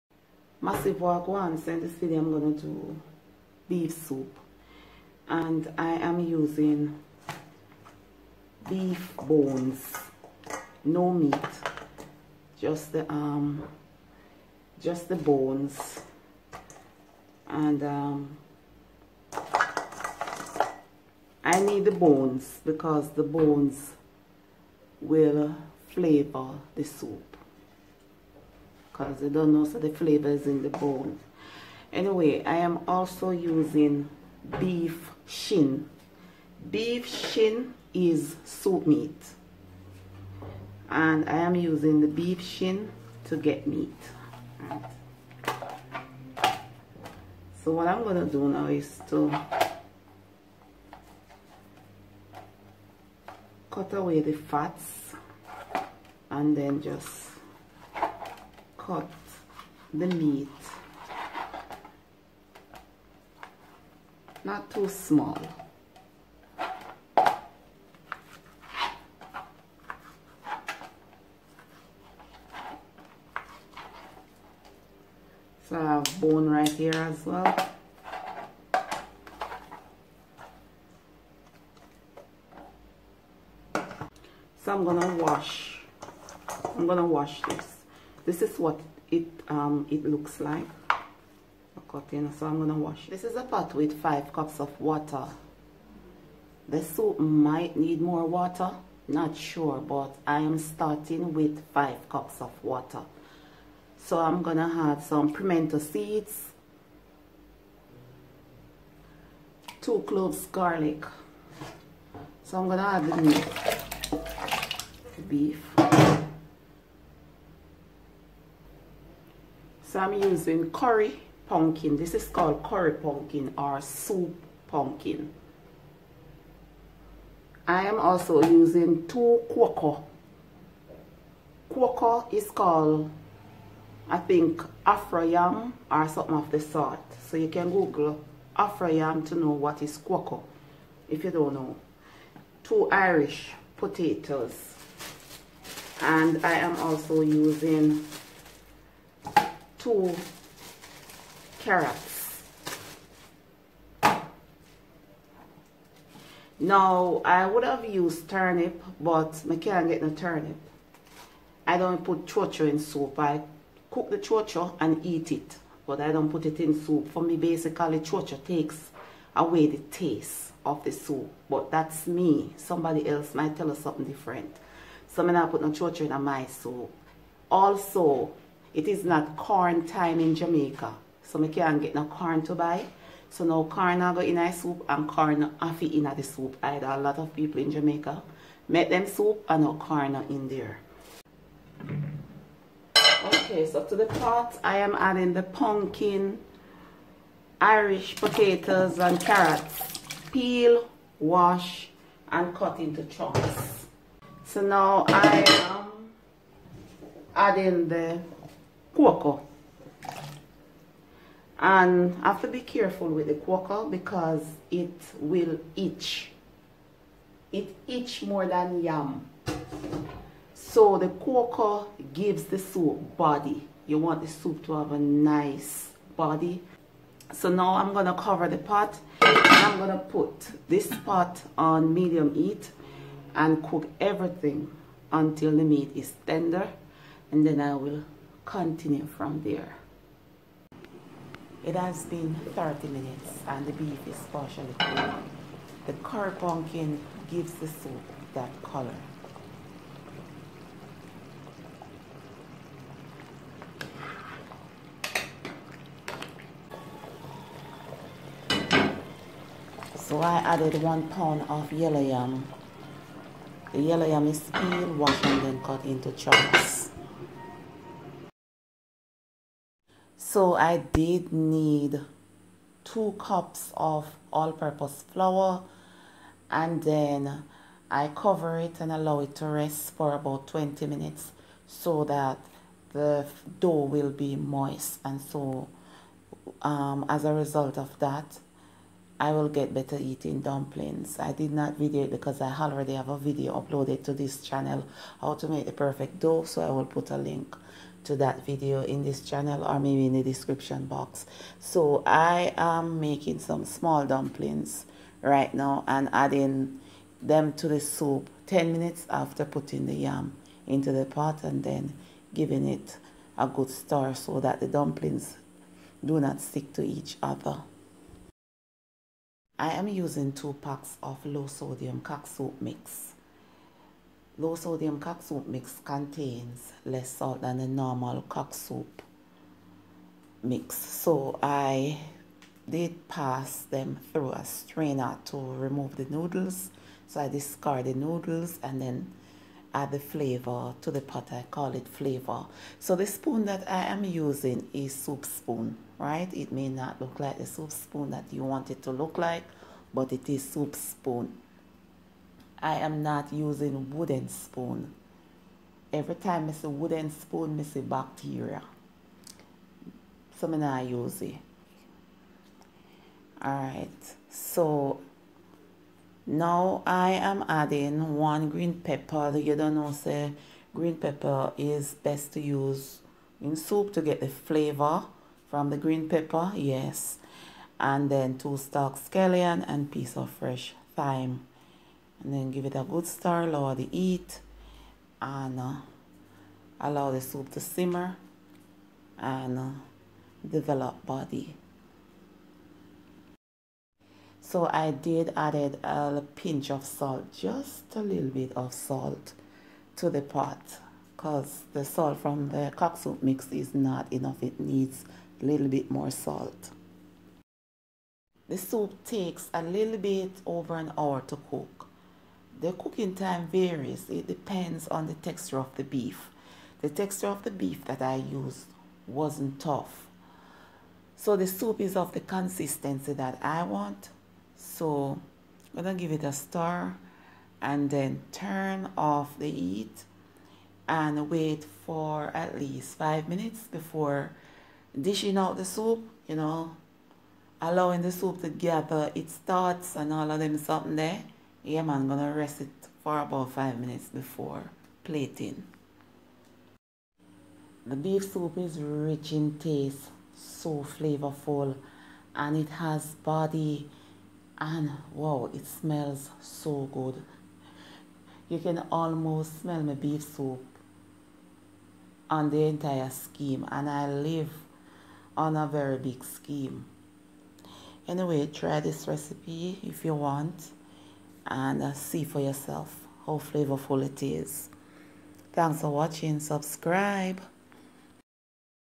Jamaican Beef Pumpkin Soup|Jamaican Saturday Soup| Jamaican Curry Pumpkin Beef Soup|YCJ
This is an easy step by step  video detailing how to cook Jamaican beef soup or pumpkin beef soup. I used beef bones as well as beef shin (shank). This is the real Jamaican Saturday soup. Watch the video, then try the recipe